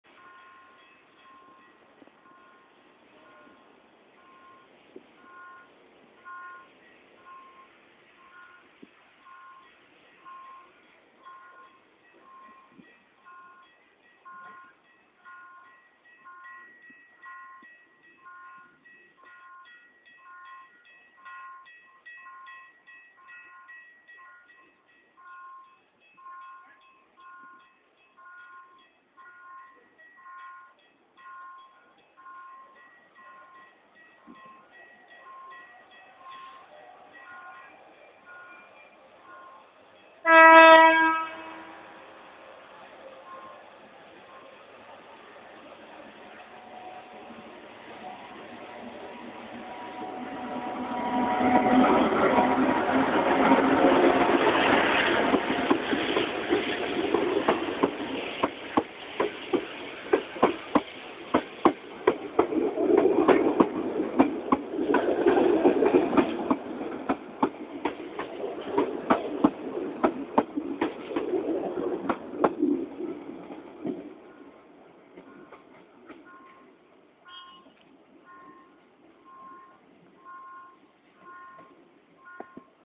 locomotora E717 de FERROVIAS
la locomotora E717 pasando por Est. Don Torcuato.